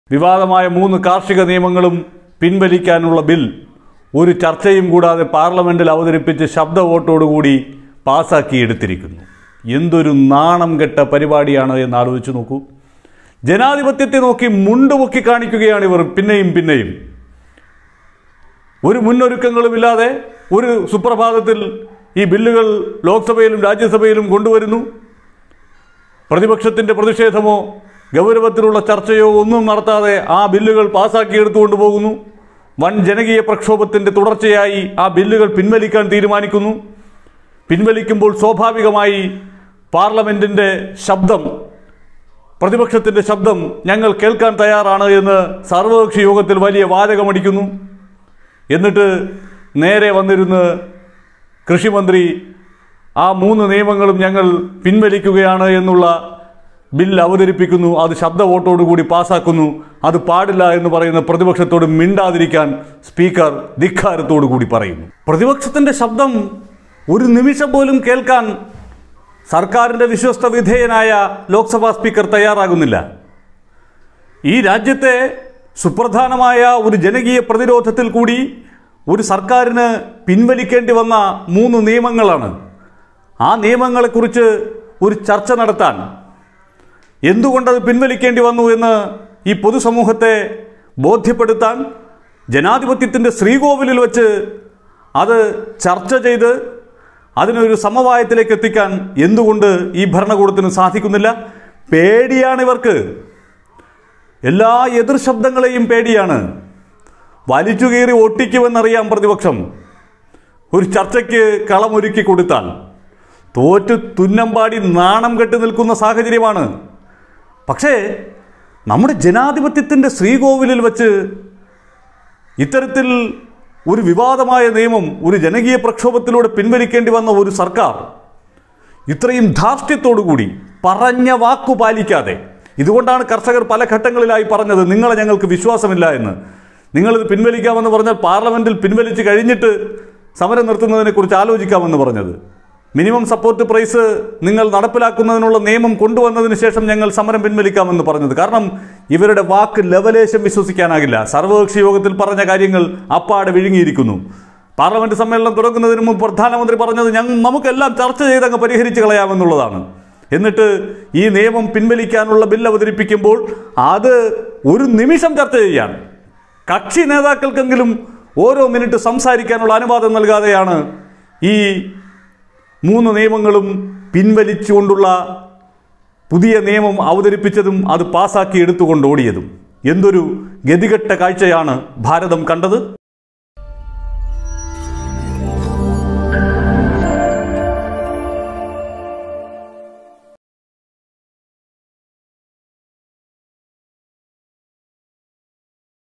വലിച്ചുകീറി ഒട്ടിക്കുമെന്ന് പേടിച്ച് പിന്‍വലിച്ചുകൊണ്ടോടുന്ന ലോക്‌സഭയിലെ ദയനീയ കാഴ്ച
Tag-Generator-Youtube-malayalam news-global